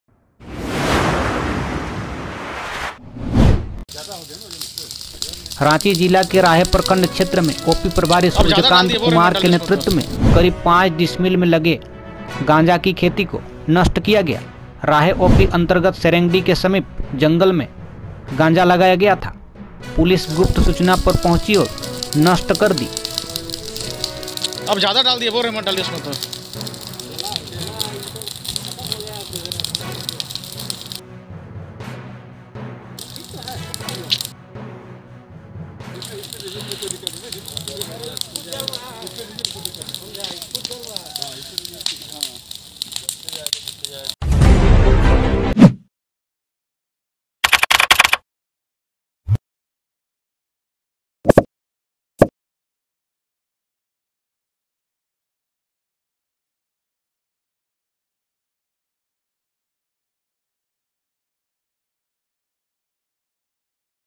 खेत में लहलहा रही गांजा का फसल को पुलिस ने किया न'ष्ट
खेत में लहलहा रही गांजा का फसल को पुलिस ने किया न'ष्टफेसबुक